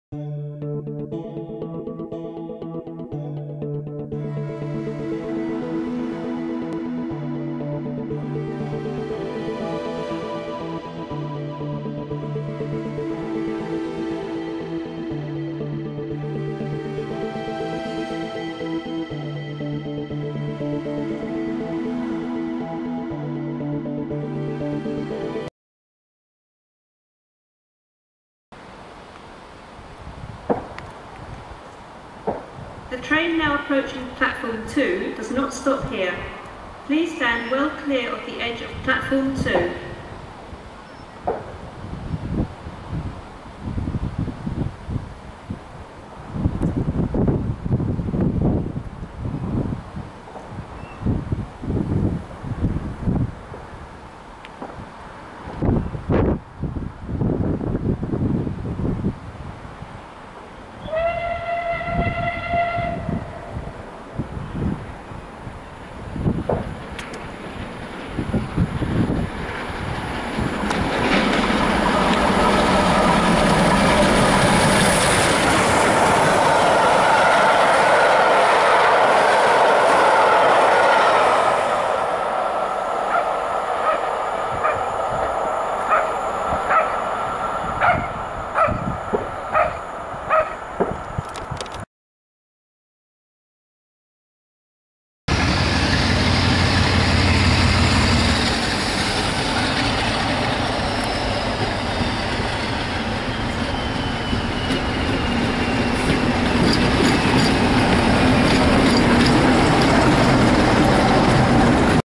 35018 British India Line Hauls The Nigel Dobbing Memorial Train Through East Midlands Parkway
A trip out the house today saw me go to East Midlands Parkway to see Merchant Navy 35018 British India Line, a locomotive i;ve been dying to see since its return to steam last year from restoration

Here we see 35018 British India Line accelerating through East Midlands Parkway, this was a memorial train for Nigel Dobbing who sadly passed away last year, this train ran from Kings Lynn to Melton Mowbray and back